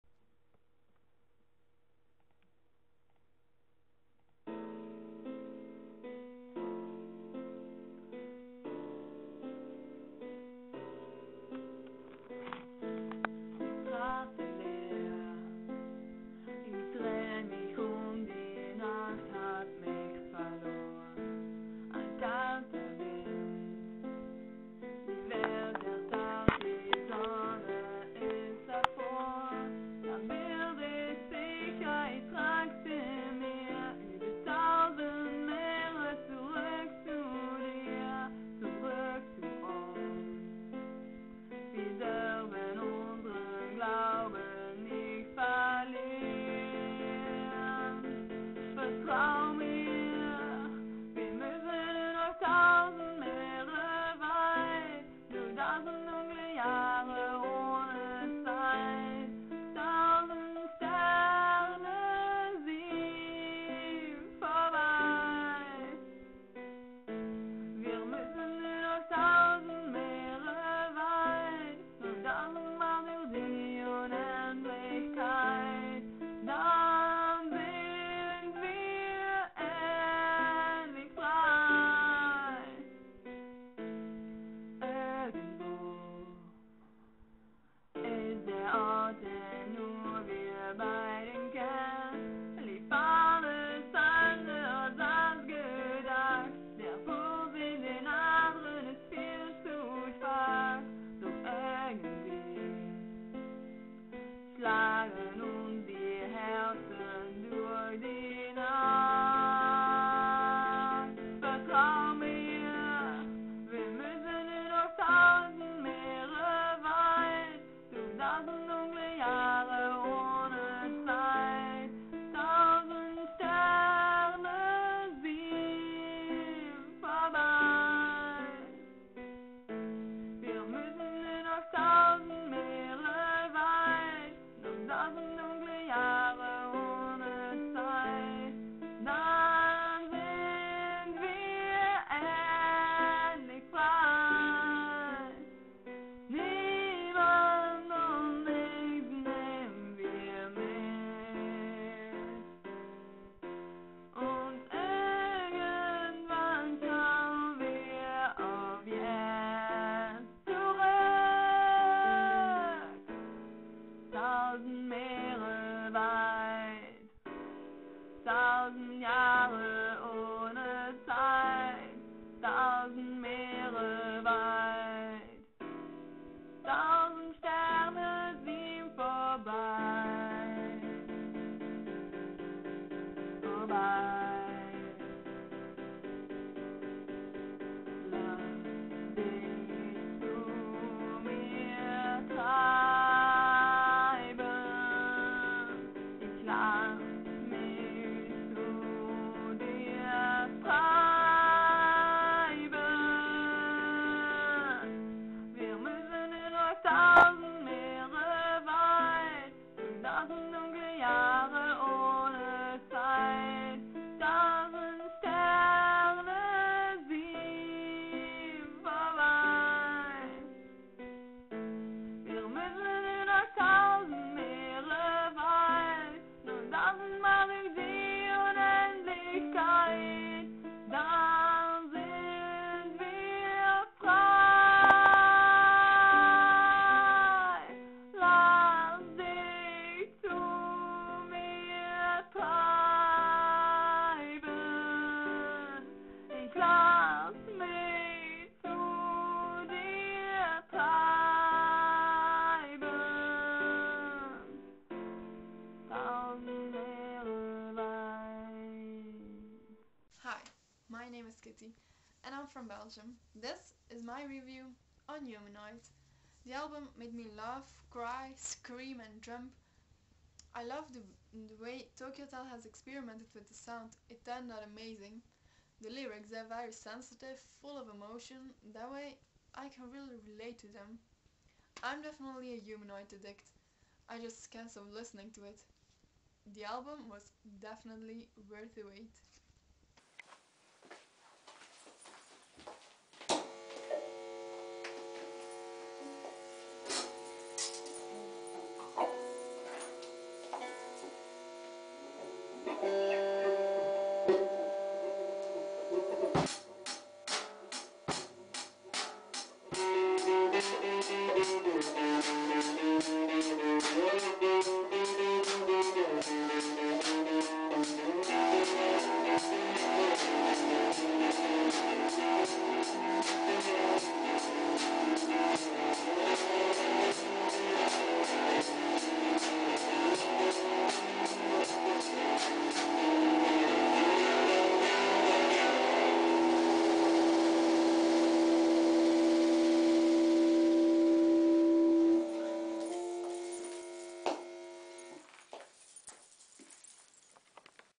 The Things I do For Tokio Hotel
The things I do for Tokio Hotel, video made for EMA's Fanaward. "I'm a fan of Tokio Hotel!"